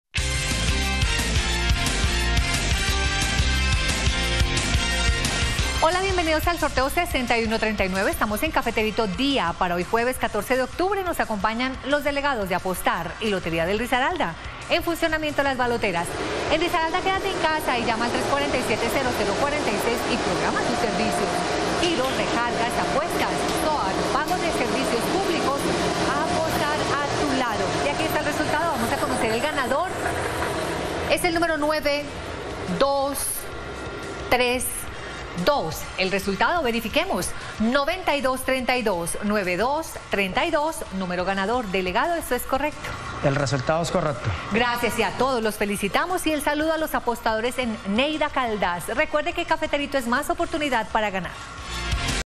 Resultado EL CAFETERITO TARDE del jueves 14 de octubre de 2021
Hola buenas tardes, te presentamos los resultados de las loterías y chances en Colombia, vamos con el Resultados del Chance Cafeterito Tarde del jueves 14 de octubre de 2021.

cafeterito tarde
cafeterito dia